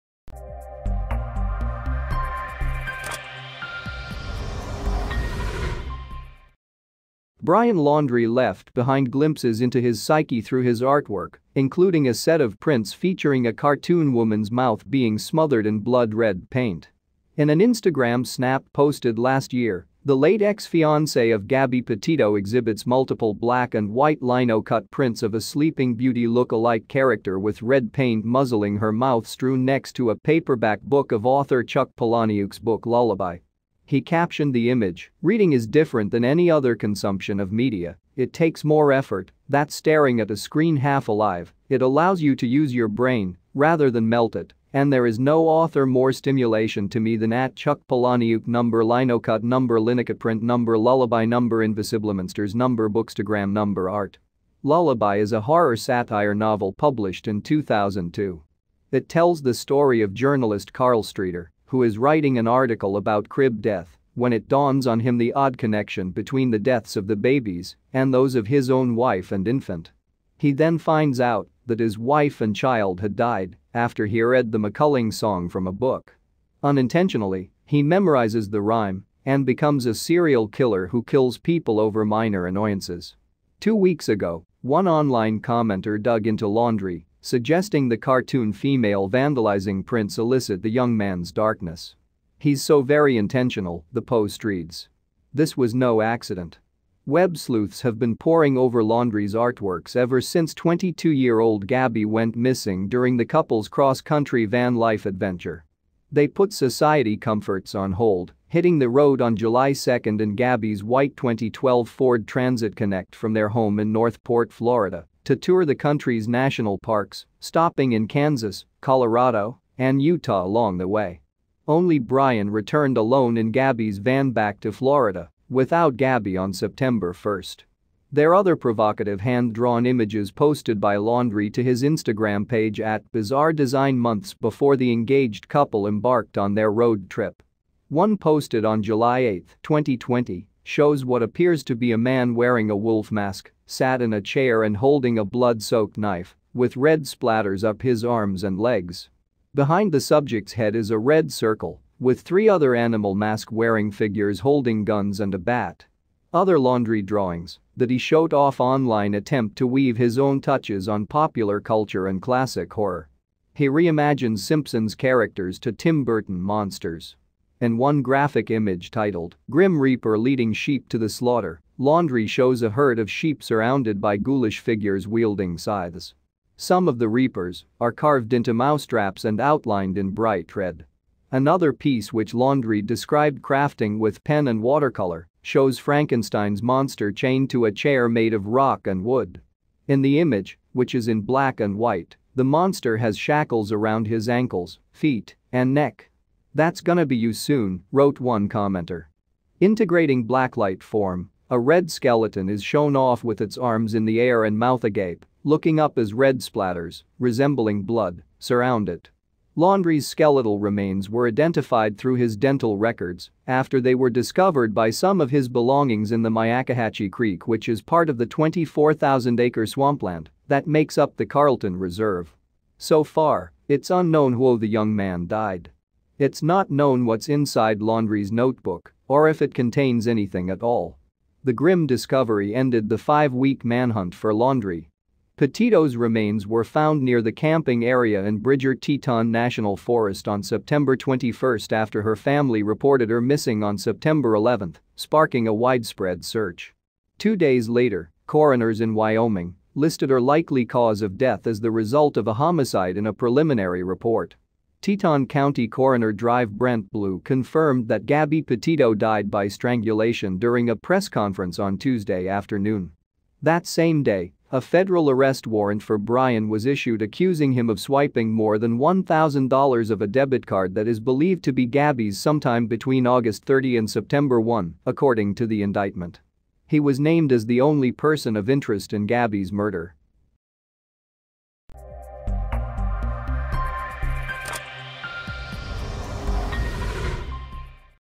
Brian Laundrie’s Instagram artwork shows ‘pictures of women being silenced’ 1
What do the pictures really mean?